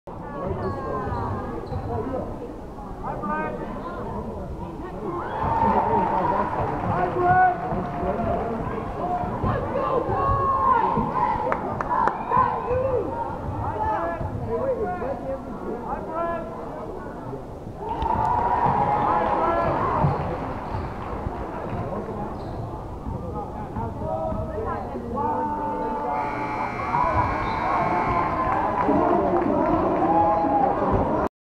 Elijah Newsome Nets 2 Free Throws And Seals The Win Against Mechanicville
Newsome Ends The Game For Cohoes. He Seals The Win With 2 Free Throws Despite The Made 3 At The Buzzer